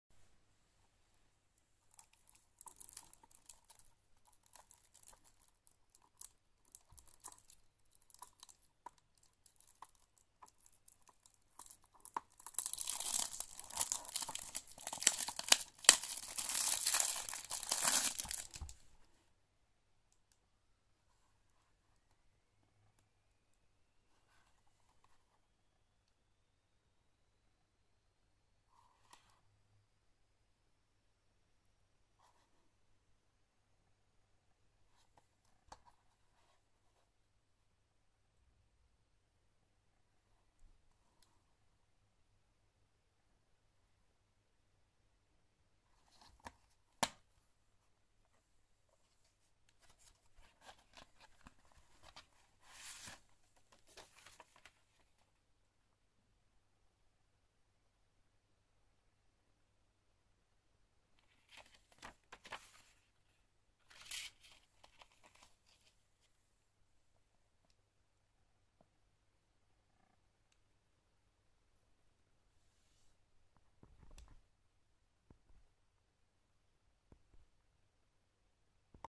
Nostalgamer Unboxing Steam World Dig A Fistful Of Dirt On Sony PlayStation Vita Limited Run Games
Nostalgamer Unboxing Steam World Dig A Fistful Of Dirt On Sony PlayStation Vita PSV Limited Run Games Region Free